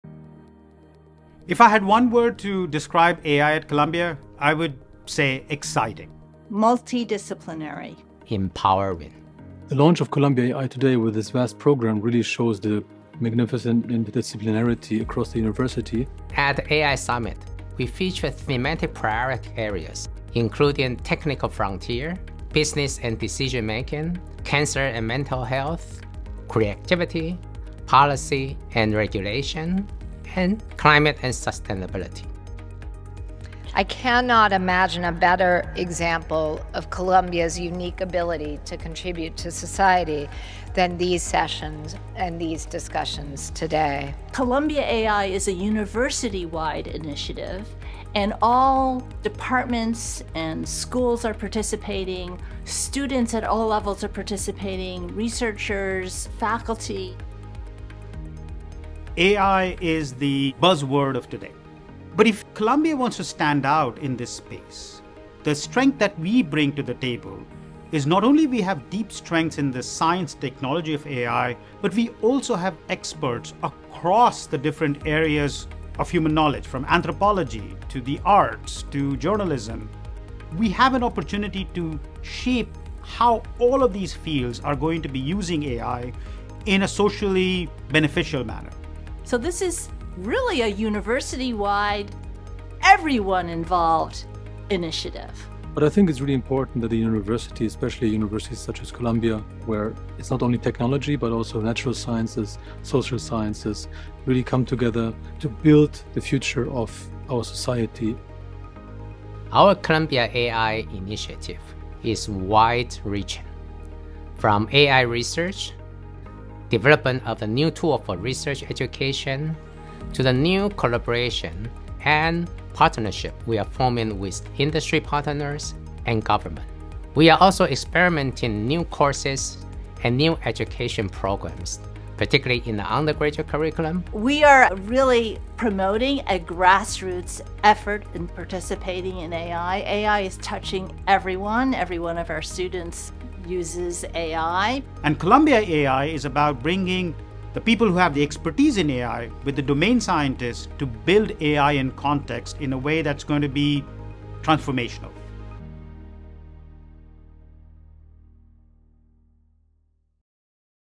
Columbia AI: A Transformational Approach to AI Through Cutting-Edge Research and Collaboration
The Columbia AI initiative – led by Garud Iyengar, director of the Data Science Institute; in partnership with Shih-Fu Chang, dean of Columbia Engineering; and Jeannette Wing, executive vice president for research – is shaping the future of artificial intelligence through cutting-edge research and interdisciplinary collaboration.

On March 4, 2025, Columbia hosted its first Columbia AI Summit, which highlighted groundbreaking projects, the integration of AI across diverse fields, and the importance of fostering innovation responsibly. With a focus on education, Columbia is preparing the next generation of AI leaders and ensuring the technology serves humanity equitably.

In this video, you will see the highlights from the summit across our three campuses, and hear from the leaders of Columbia AI.

You can watch every Columbia AI Summit lecture, panel and keynote speech by Sami Haddadin by visiting the Columbia Data Science Youtube

For more on how Columbia University is at the forefront of AI innovation,